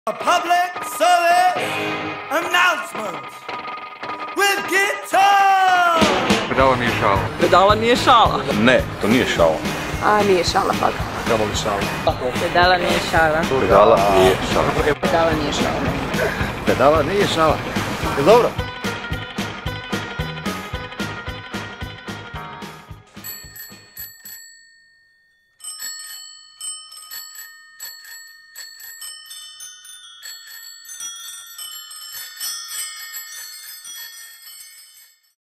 Pedala nije šala
Možda niste znali...
Kritična masa: Pedala nije šala u nedjelju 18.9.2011. ispred Mimare, Zagreb u 16h. Veseli smo, ali ozbiljno mislimo :)